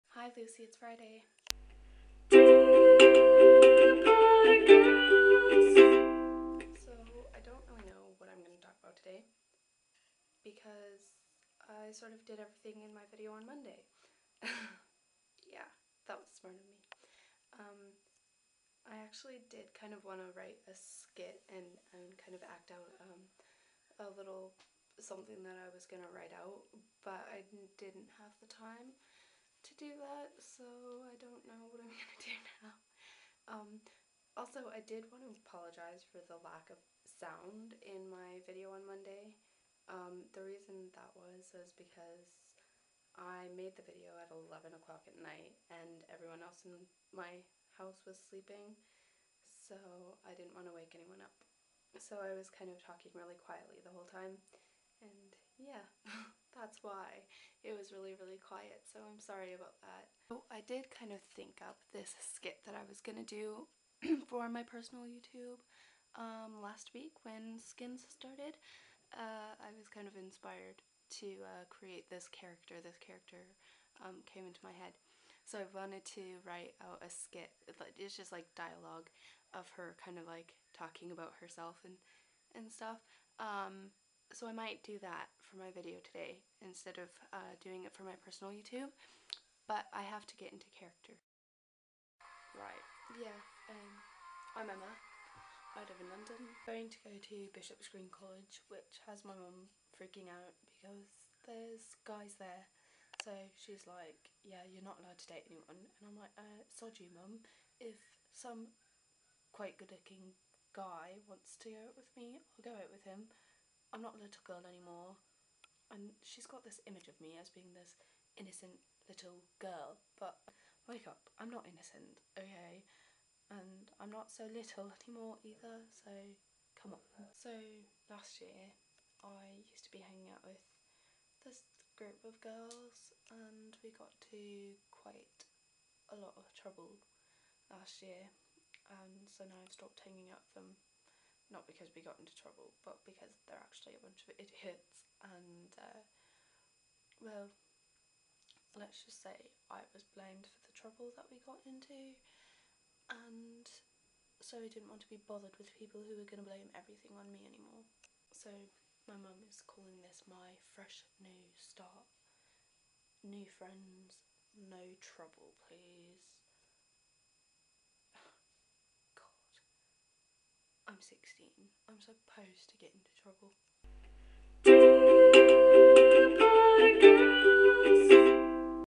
Katherine likes acting!
Katherine chats a bit and then acts out a little dialouge.